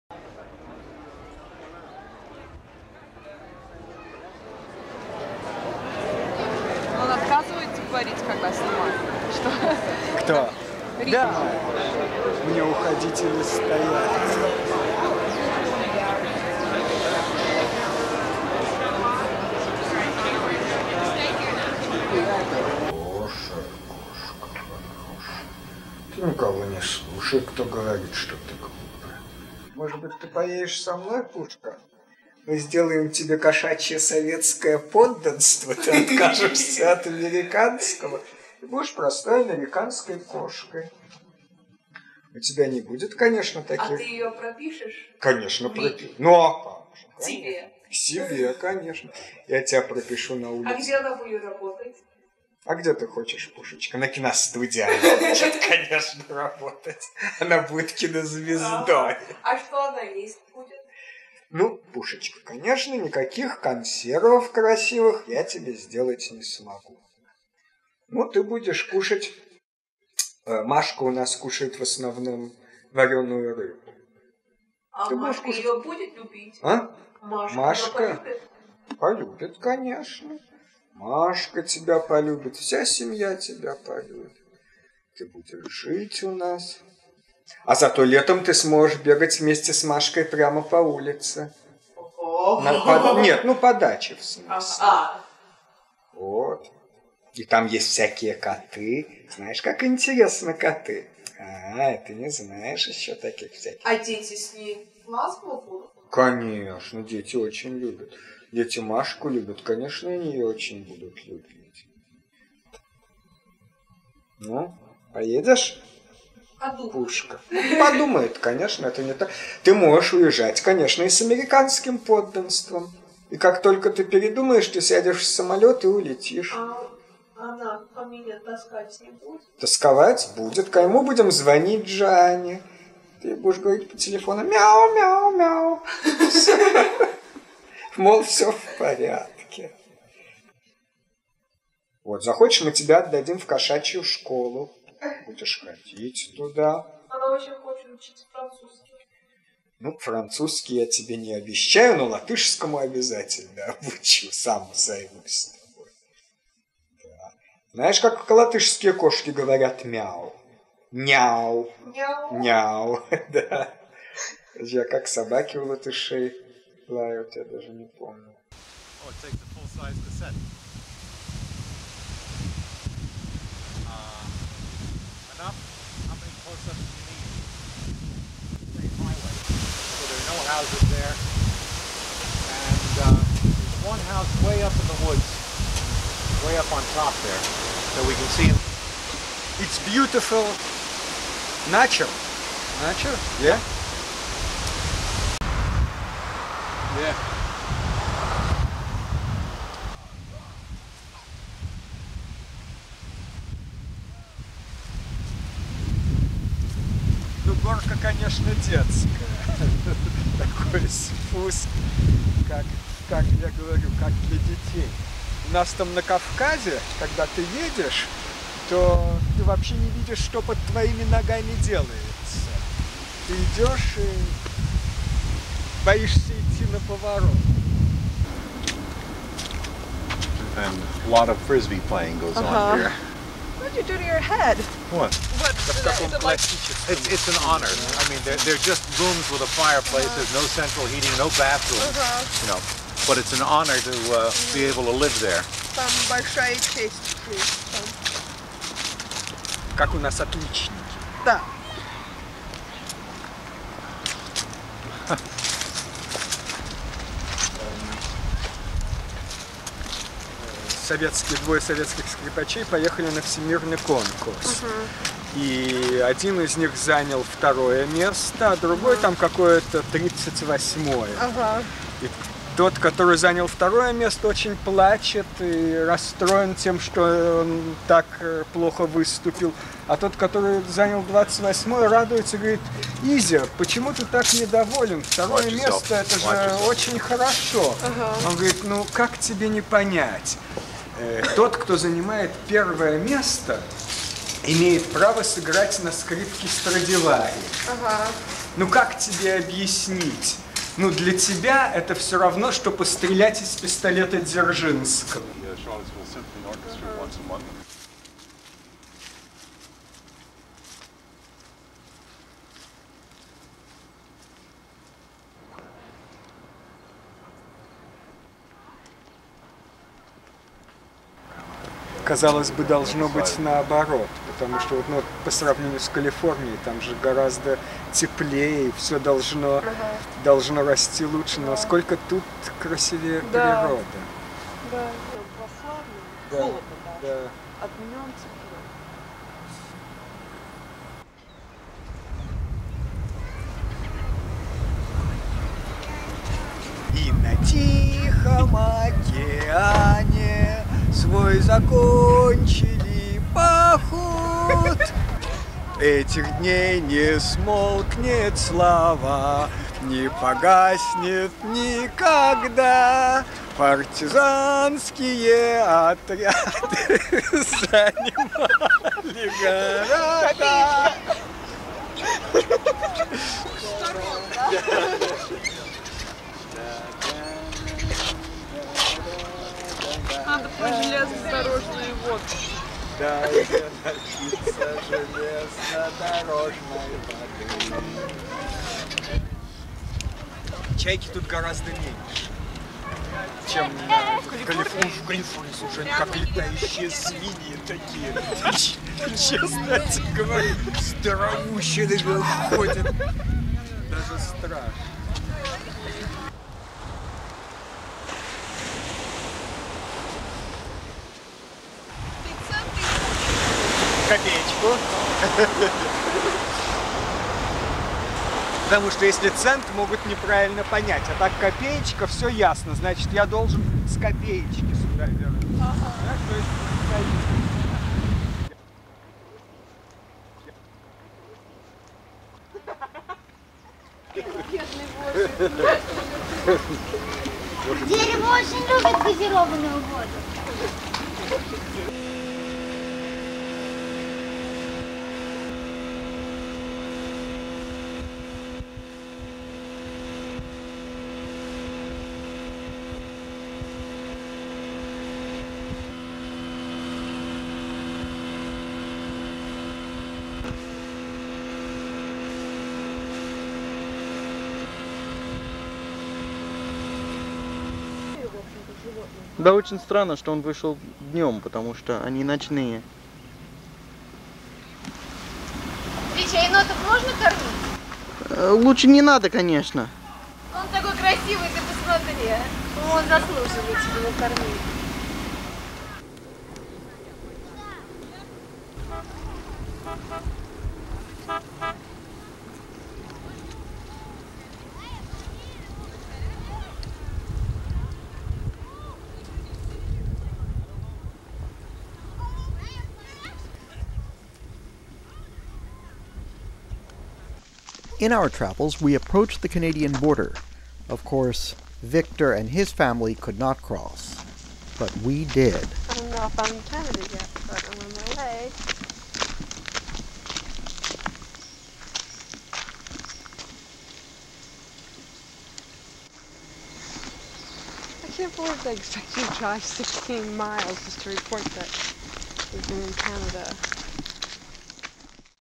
Victor Rosenberg in America 1988-89
Victor Rosenberg was one of the first Soviet citizens to visit the U.S. as a normal tourist.